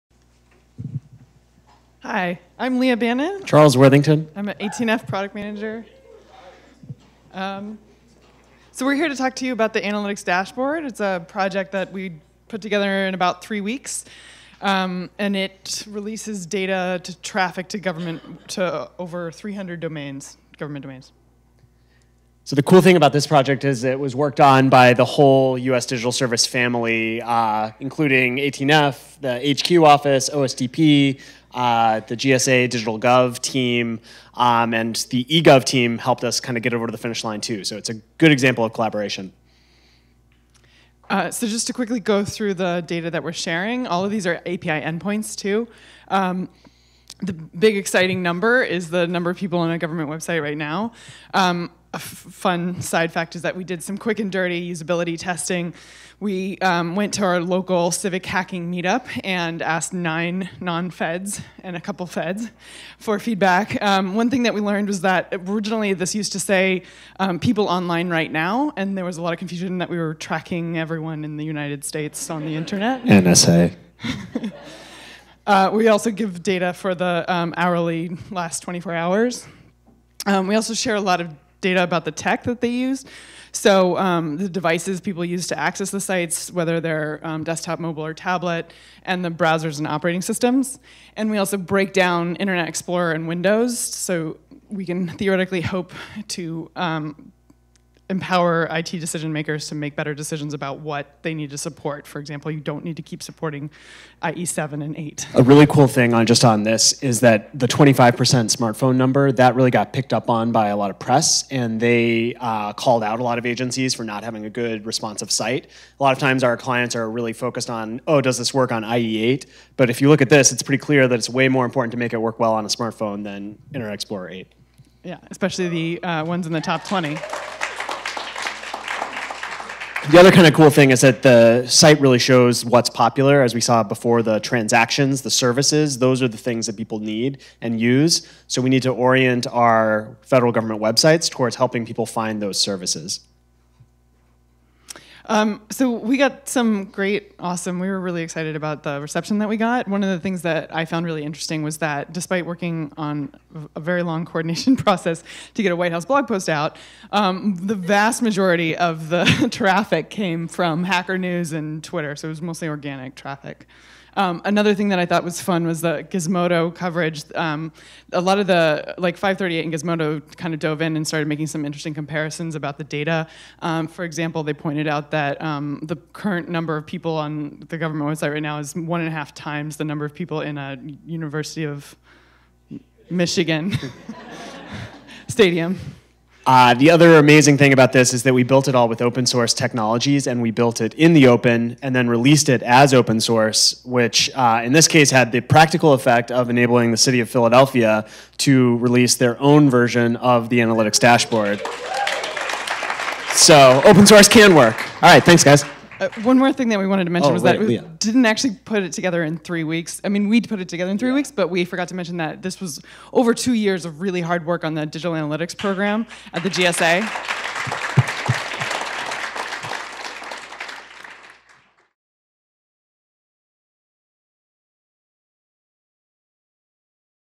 Building an Analytics Dashboard for the Entire Government
18F's Leah Bannon and Charles Worthington of the U.S. Digital Service explain the new a website that tracks real-time traffic to .gov domains. This presentation was part of the first moveForward(100); event.